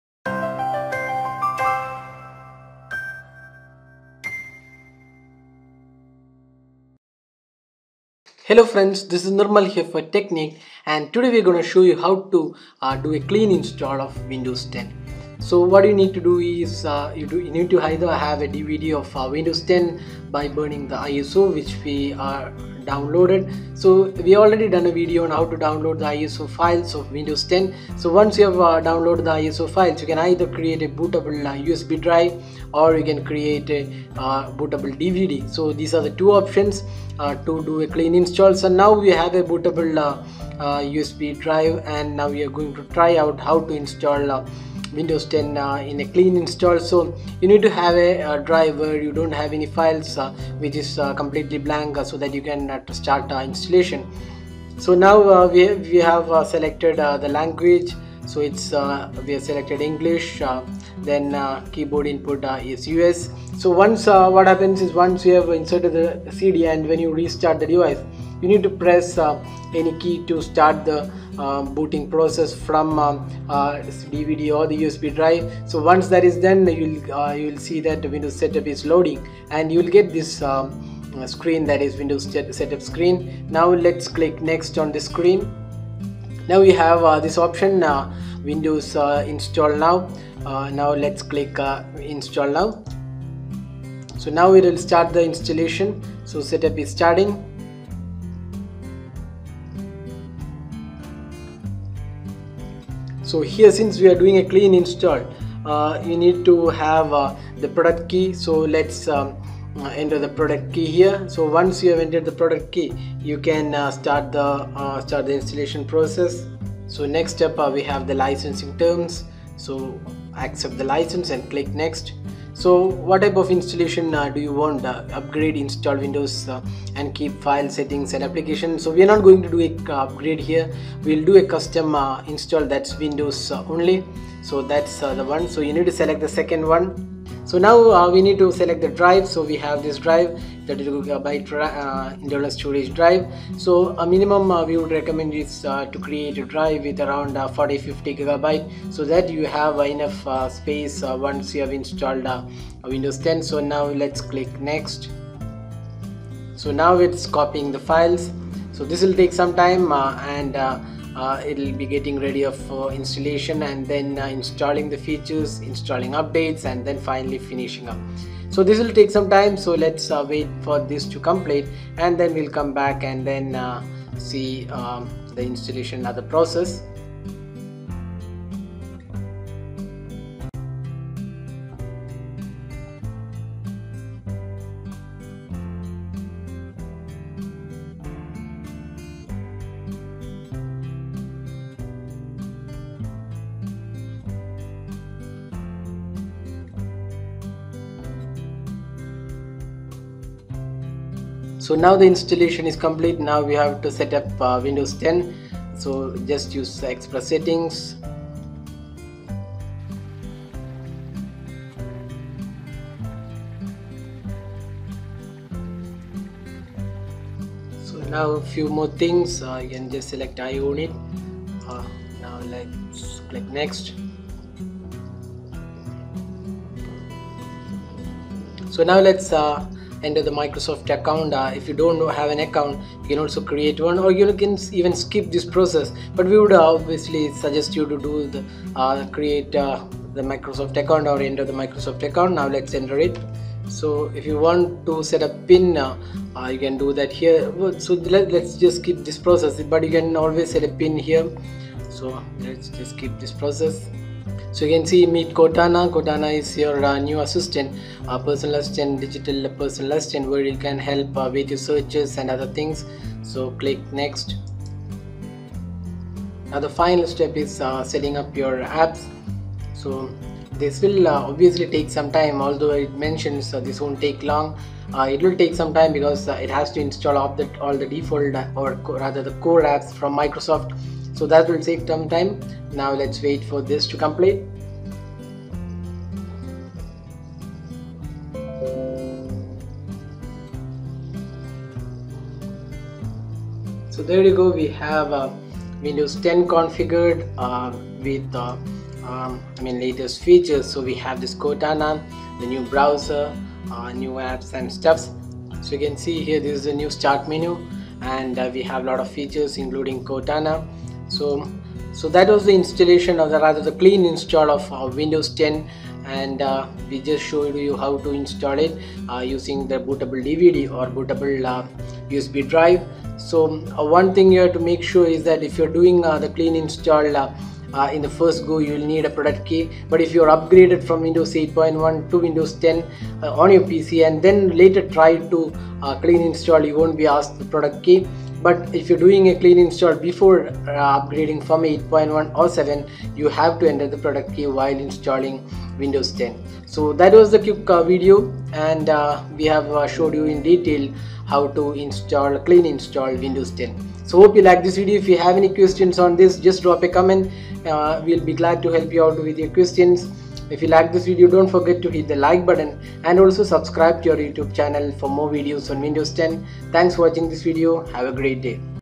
How to Clean Install Windows 10 | Techniqued
Step by step process on how to perform a clean install of Windows 10. You can easily install Windows 10 on your PC following these steps.
How to Clean Install Windows 10?